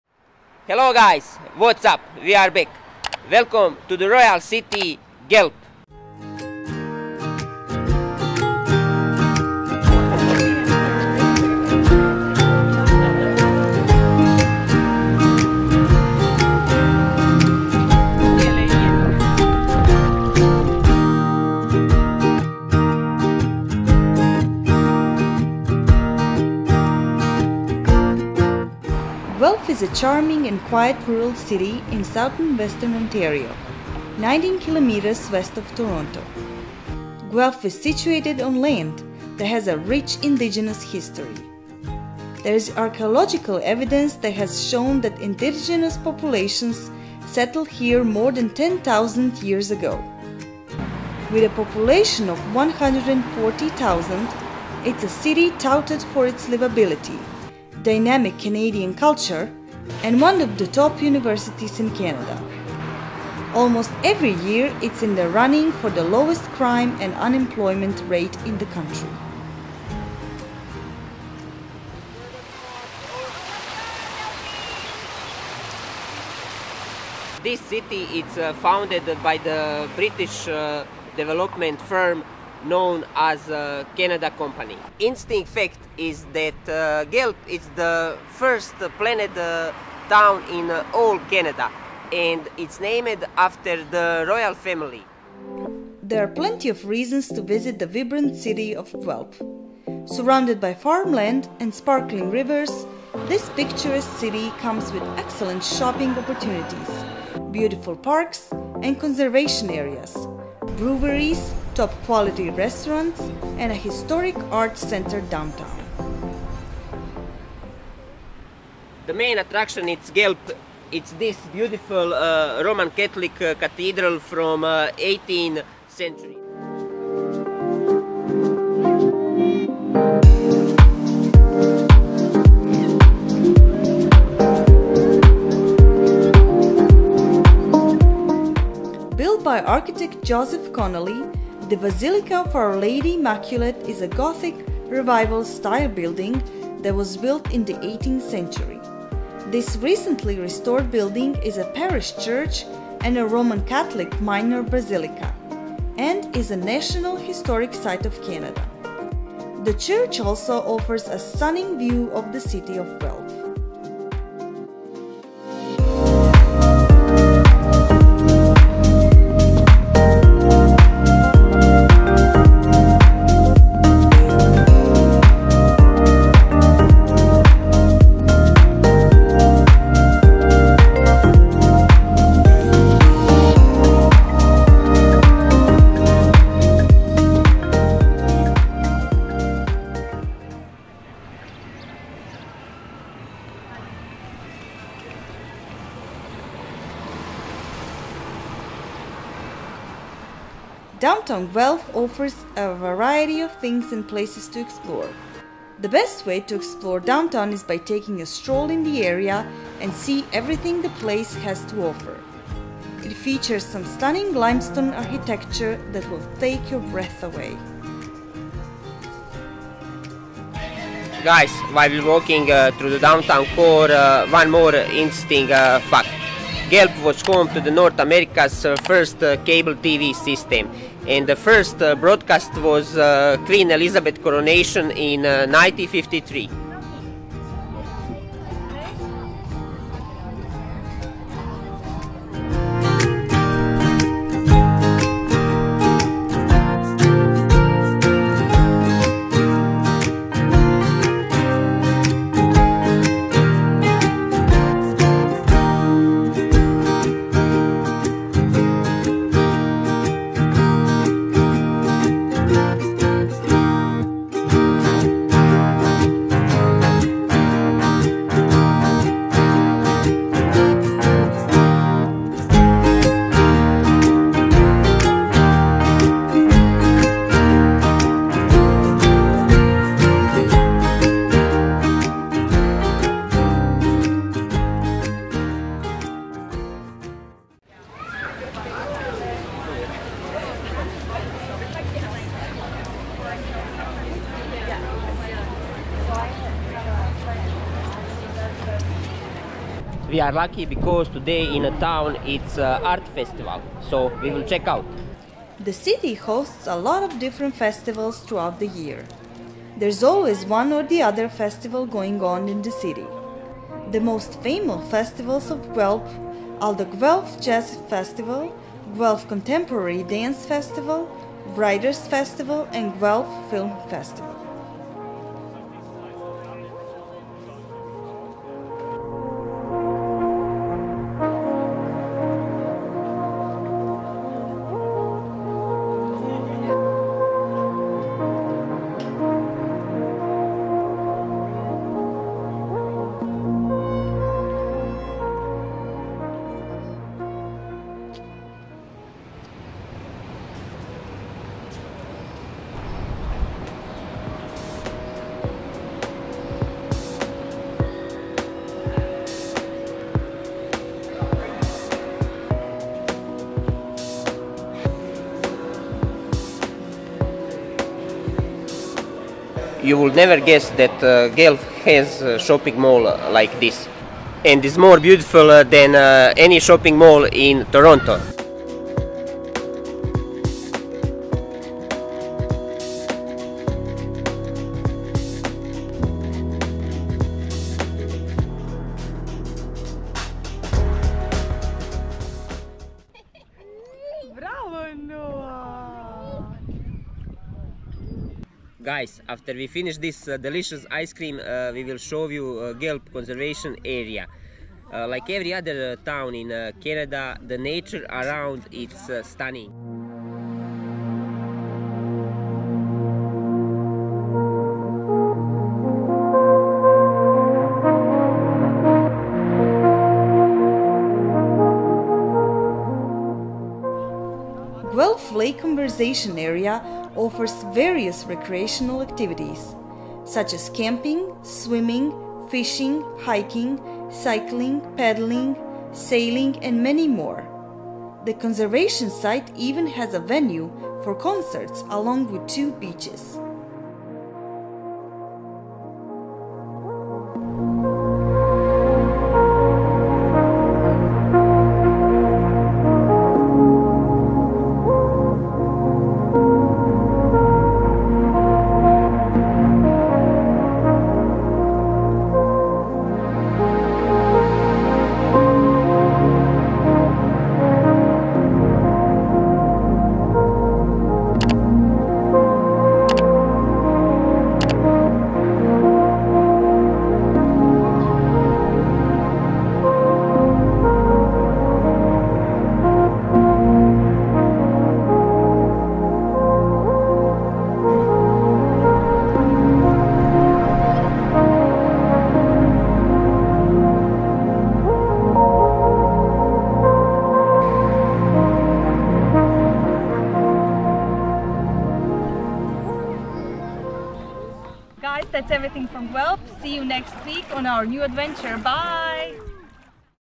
We Visited Guelph in Ontario - The Most Livable Place in Canada
Hello guys from Ontario!
Today we will visit one of the most livable places in Canada - Guelph in Ontario. Guelph is the first planned town in Canada, named after the royal family. Except for the vast history, the city of Guelph is surrounded by great nature and some of the best sceneries in Ontario. Before we stroll through the downtown of Guelph, we will visit the most recognizable landmark in the city the Gothic Revival style Basilica from the 18th century. After the basilica and downtown for the end, we will visit the Guelph Lake Conservation area and spend the afternoon on one of the beautiful beaches that this area offers.